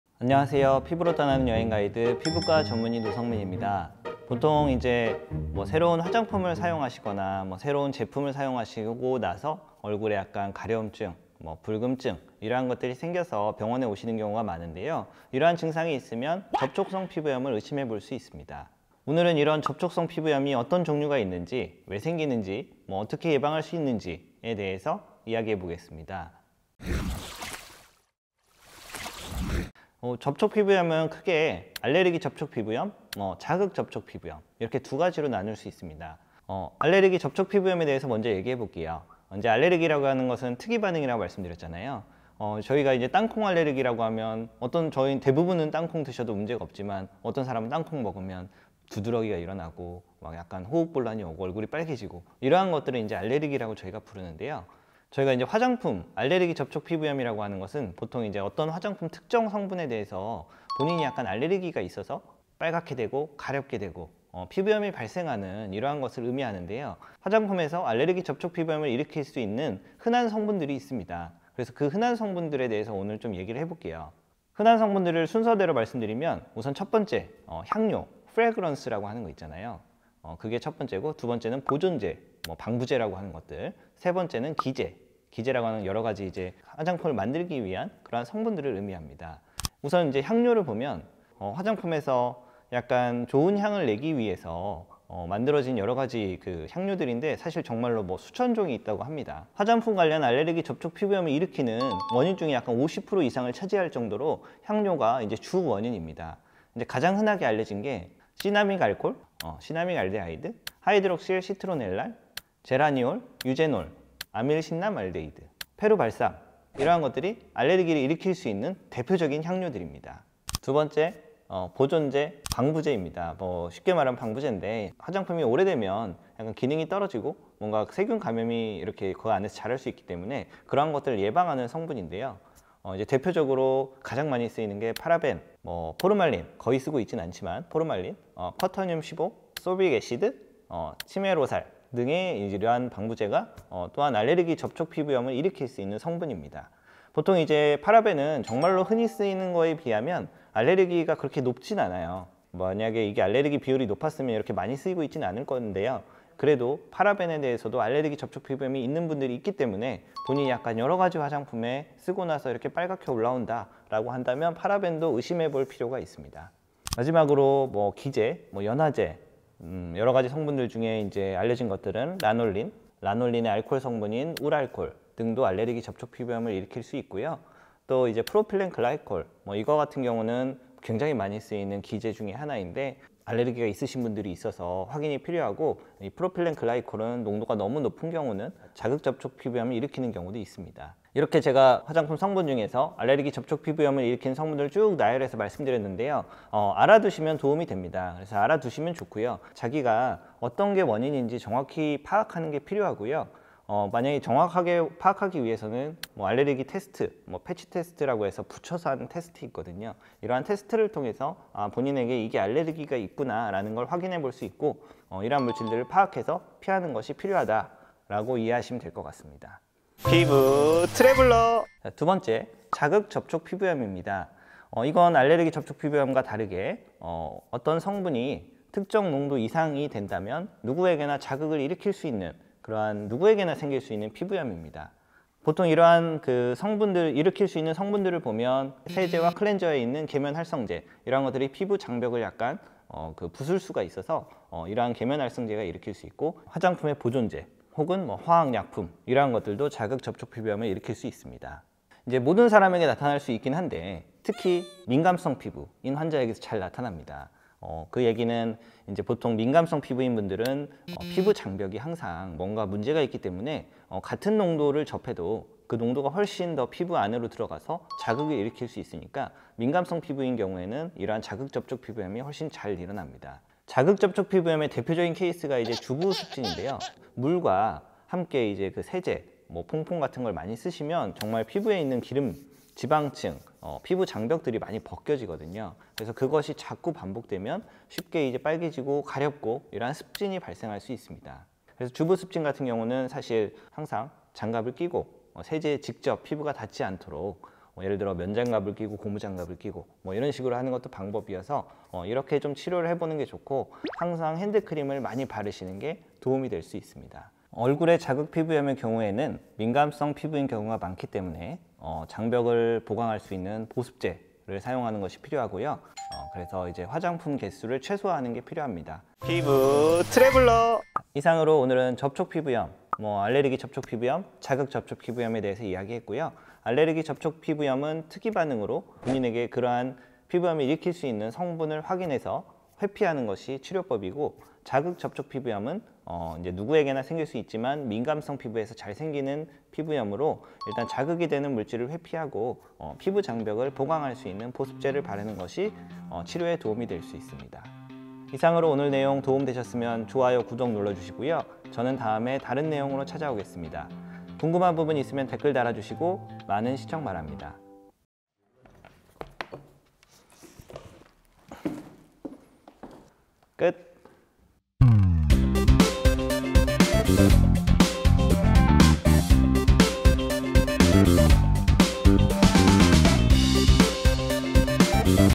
(ENG) 피부 가려움증과 붉음증이 있다면!? 😱ㅣ접촉성 피부염의 종류와 치료법👨🏻‍⚕️｜피부 트래블러 - 피부과전문의 노성민
안녕하세요 피부로 떠나는 여행 가이드 피부과 전문의 노성민입니다👨 ⚕
오늘은 '접촉성 피부염의 종류와 치료법' 에 대해 알아보고자 합니다🙌

여러분의 피부염에 조금이나마 도움이 되길 바라며
궁금한 점은 댓글로 적어주세요⌨

앞으로 피부로 떠나는 여행가이드로!🌏✈
재밌고 유익한 정보 드리도록 노력 하겠습니다~!
그럼 피부로 여행을 떠나러  함께 가보실까요~?!